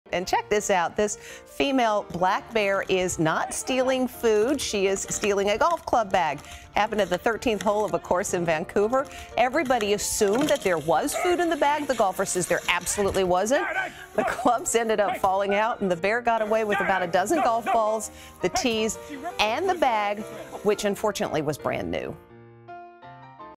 Naughty Bear Steals Brand New Golf Bag
A female black bear in Vancouver, Canada recently was in search of food. Much to some nearby golfers' dismay, she made off with a brand new golf bag instead.